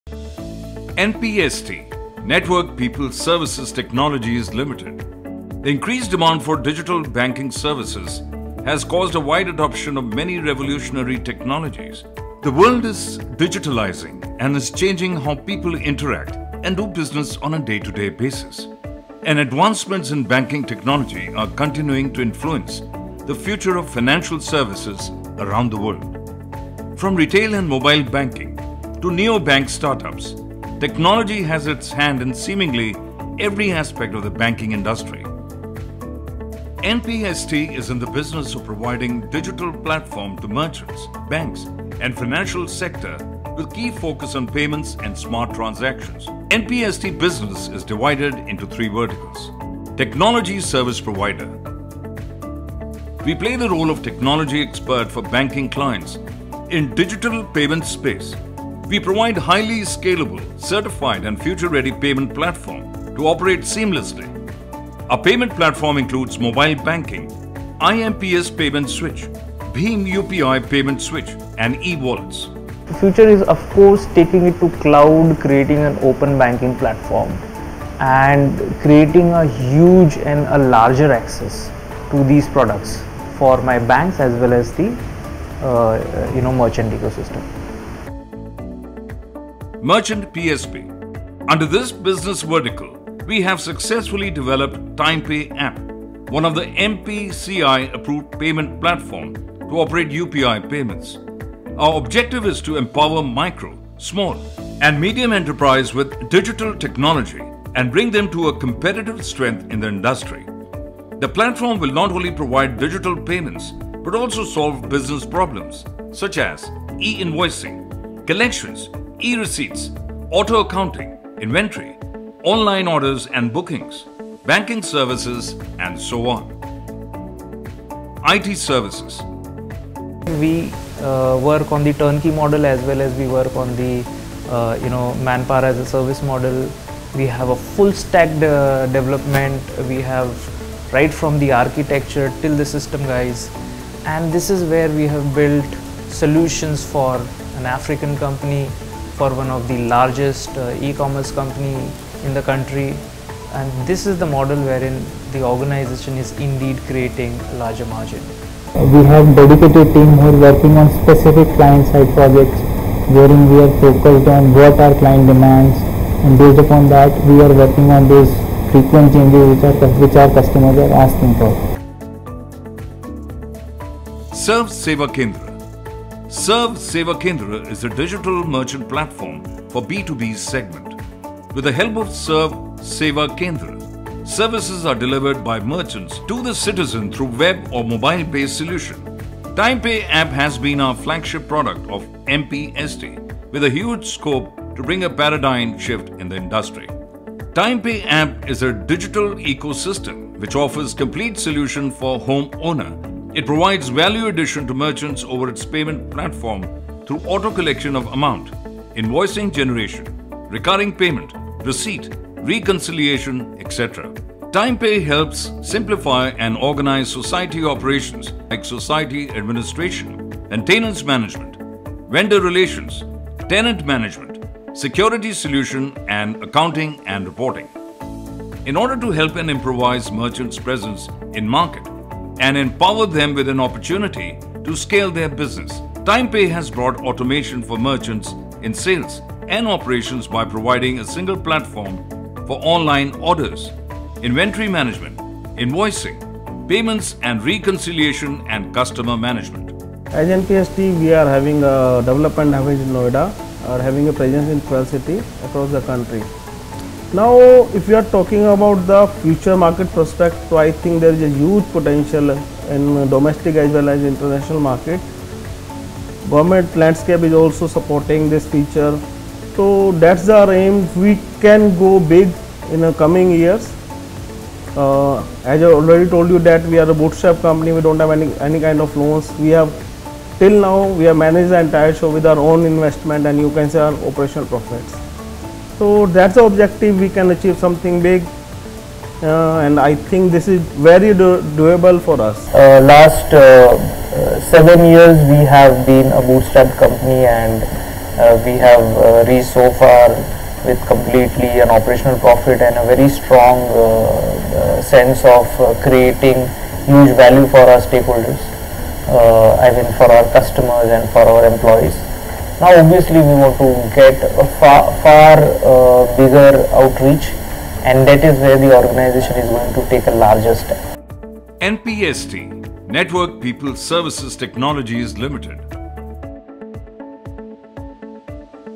Network People Services Technologies Ltd - Short Film
NPST is in the business of providing Digital Payments solution to banks, financial institutions, and Merchants. Our Primary focus being Financial Technology, we have built a multi-layered solution for the cashless economy.